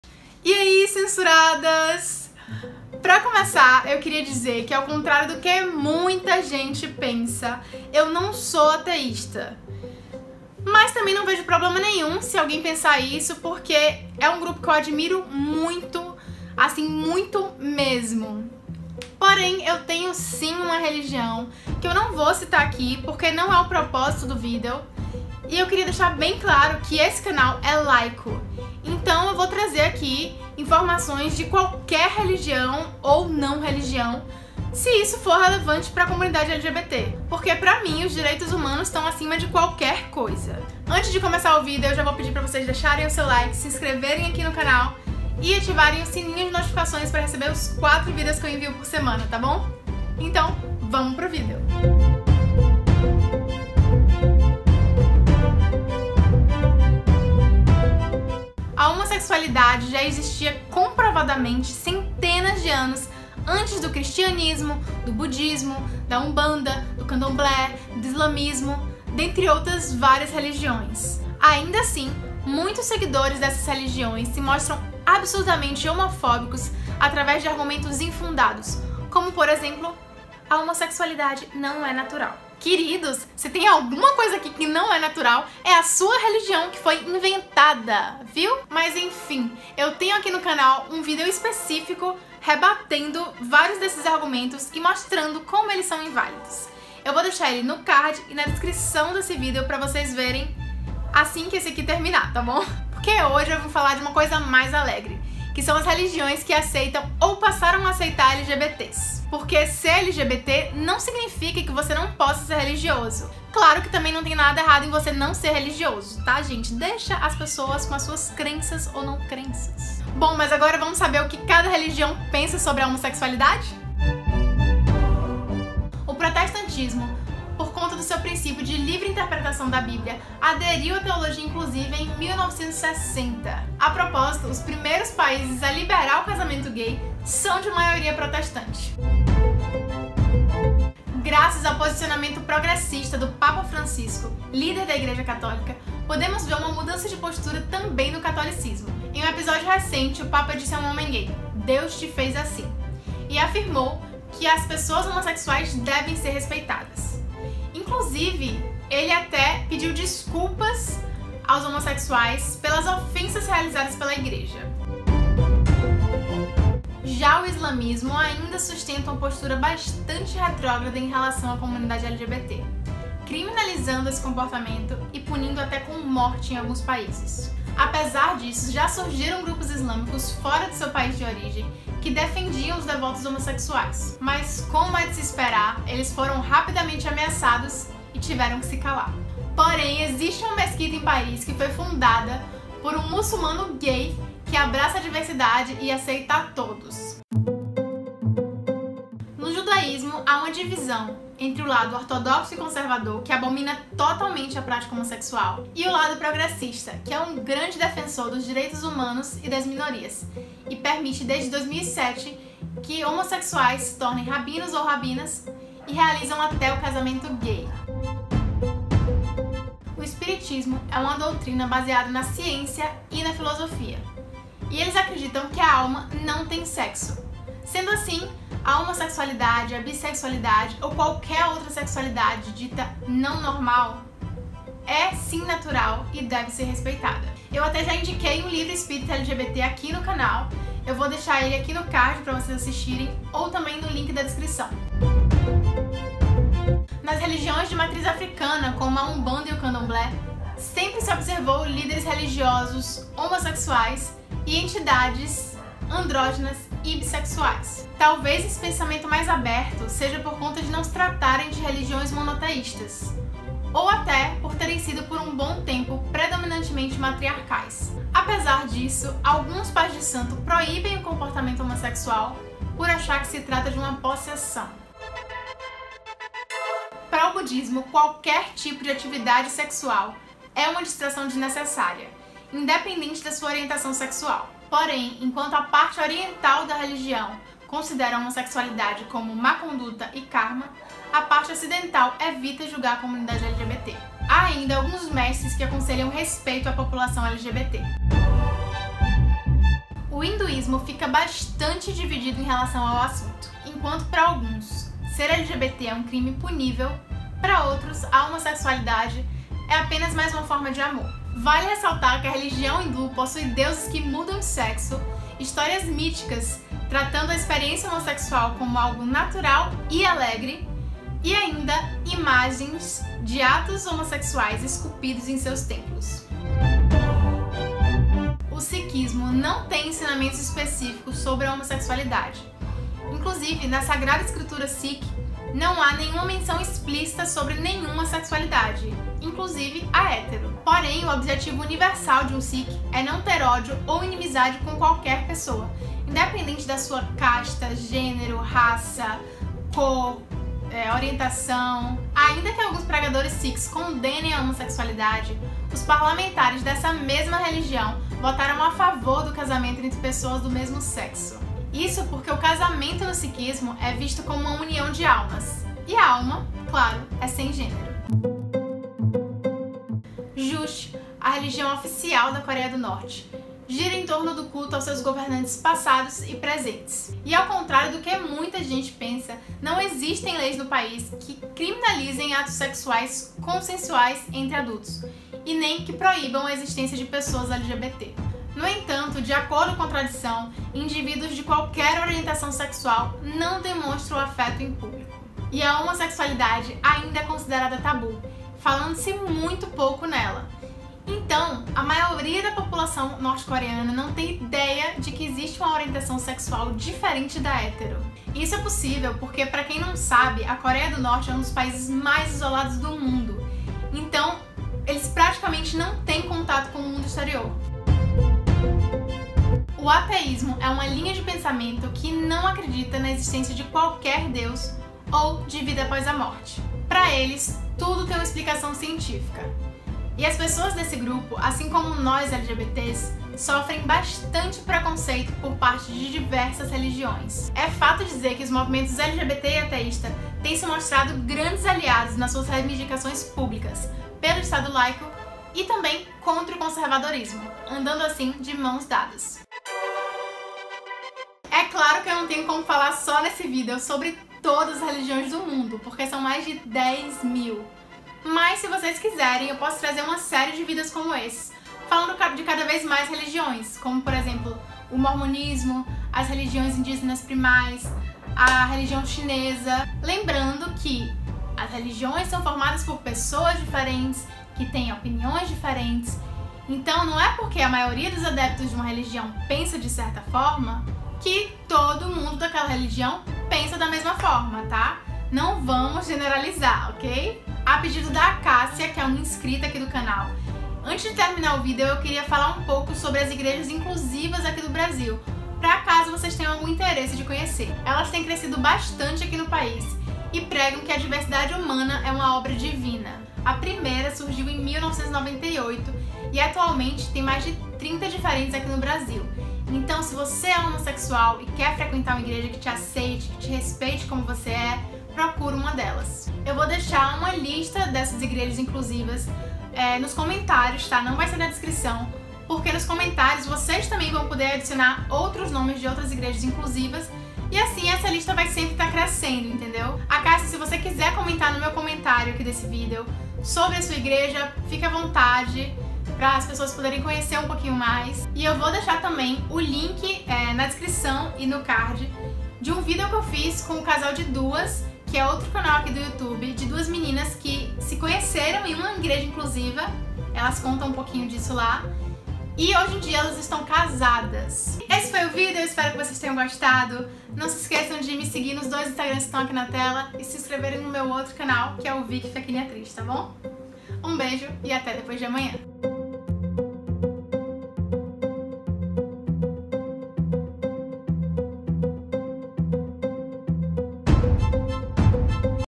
O que as religiões pensam dos LGBTs?
Descubra como cada uma das maiores crenças do mundo e do Brasil tratam a questão da população LGBT. Será que diversidade e fé podem coexistir?

Vicky: @vickyfechine
Canal: @canalcensuradas

Quer apoiar meu trabalho?

Mande algo pra mim:
CEP: 22793971
Rio de Janeiro - RJ
Victória Fechine